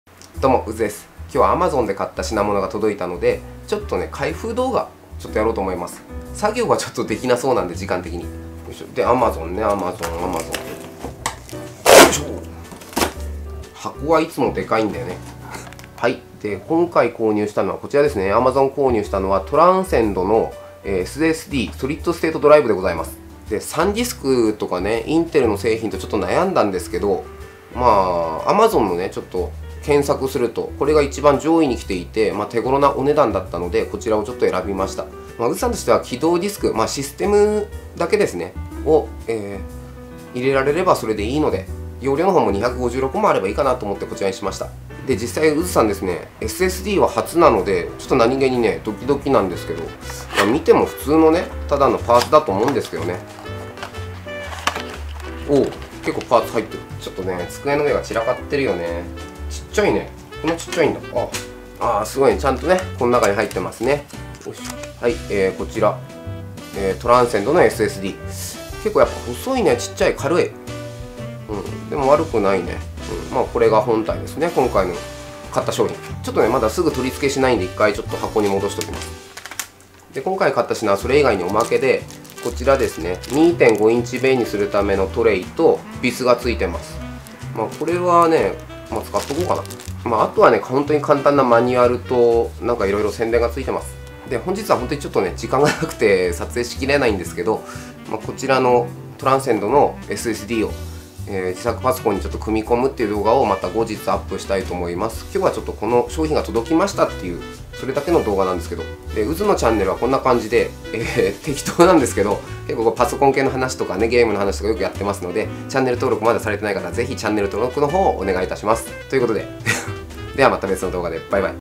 【自作PC】SSDをやっと購入しました。今回は開封動画！TranscendのSSD370
LINE＠のID「@PXY5618R」
・ウズのTwitter↓